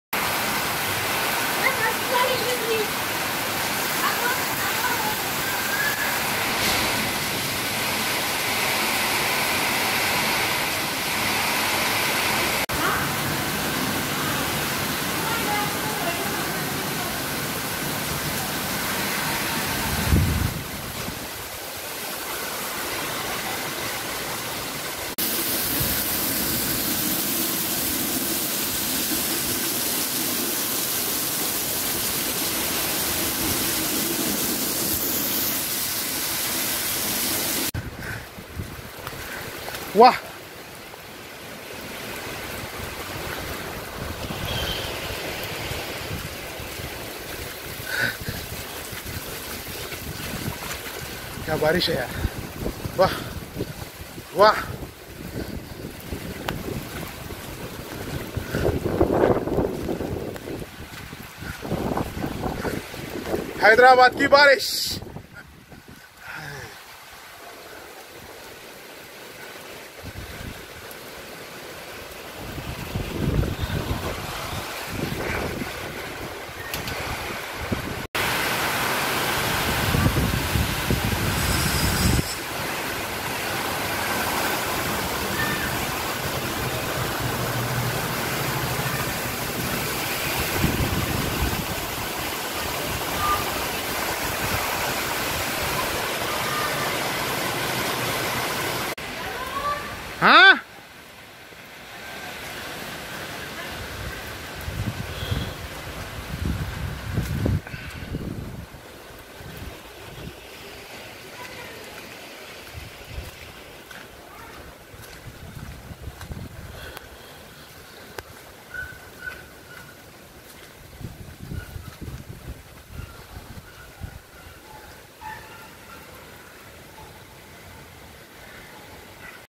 'Hyderabad mai Baarish ka swagat hai' 'Barsaat' Rainfall Rain Hyderabad Welcome 18-06-2020
Hyderabad welcomes much needed Rain.